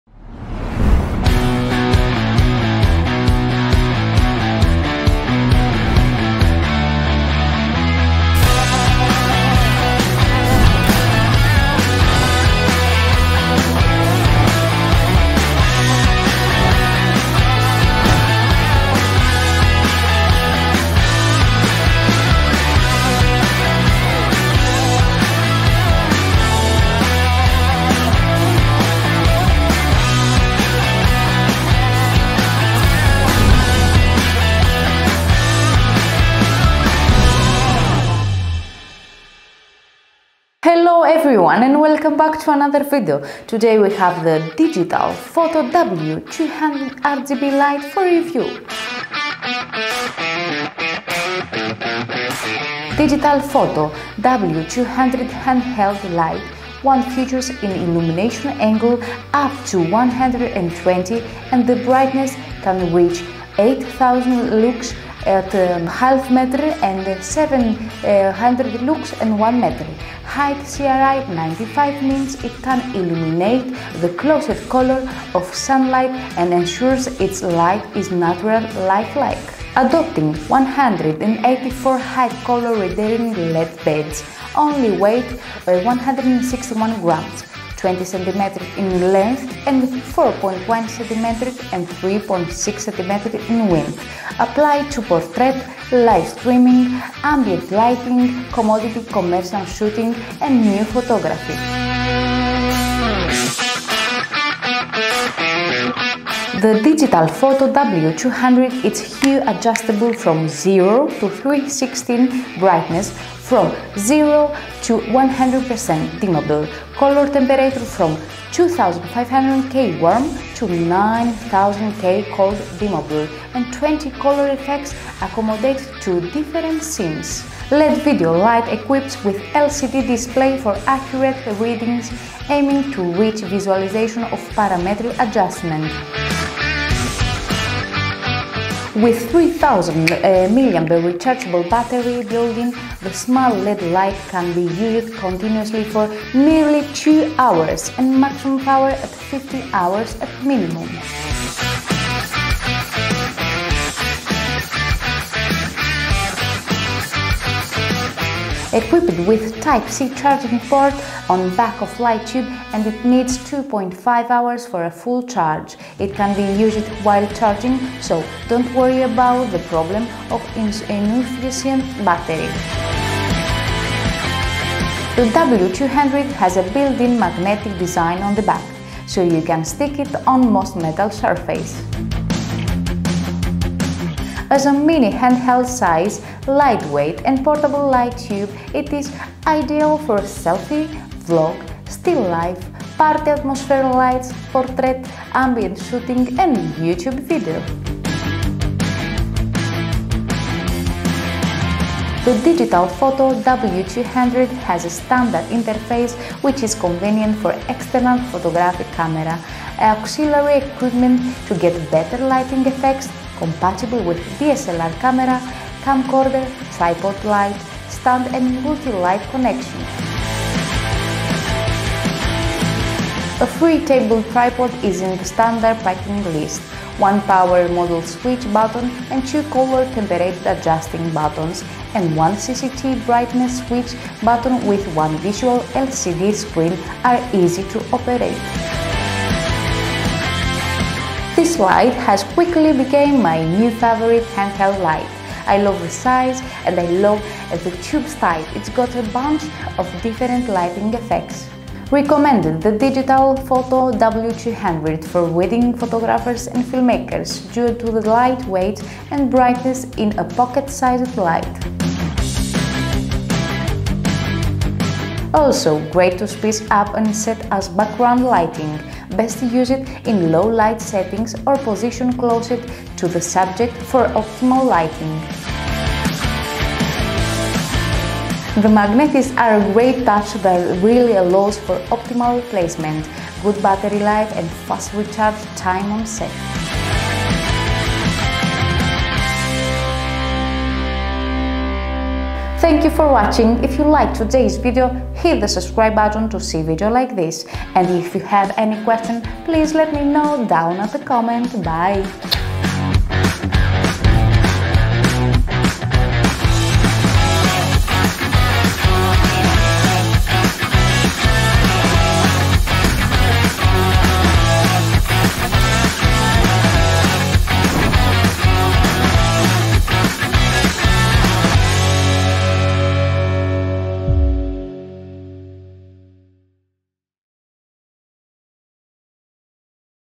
Digitalfoto W200 Rgb - Review
DigitalFoto W200 handheld Light wand features in illumination angle up to 120°, and the brightness can reach 800LUX(0.5m) and 700lux(1m).
The W200 rgb has 20 color effects accommodate to different scenes.
With 3000mAh rechargeable battery built in, the small led light can be used continuously for nearly 2 hours at maximum power and 15 hours at minimum. Equipped with Type C charging port on back of light tube and it needs 2.5 hours for a full charge.

► ☕️ Feel free to donate on buymeacoffee, to help us keep creating better content for you.